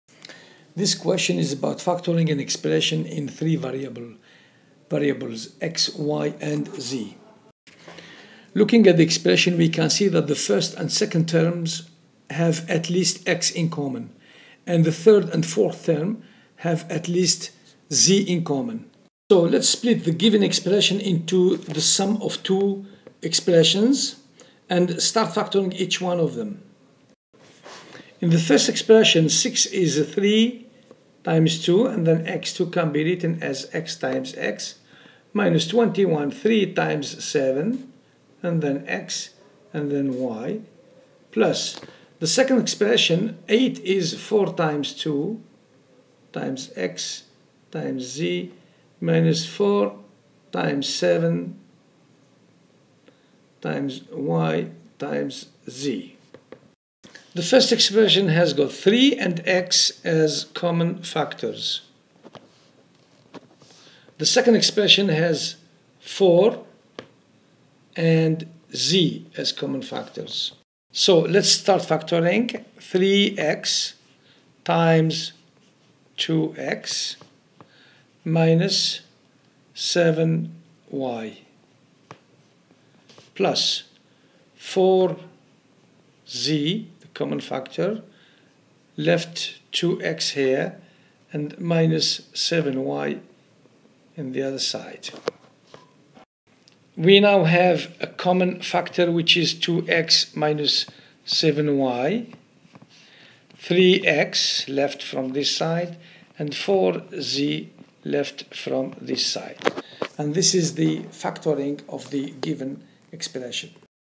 Factoring Expressions
In this video, an expression with three variables is factored by:

1 - splitting the expression into two major expressions
2 - factoring by reverse distribution (common factor)
3 - factoring using a whole expression as a common factor.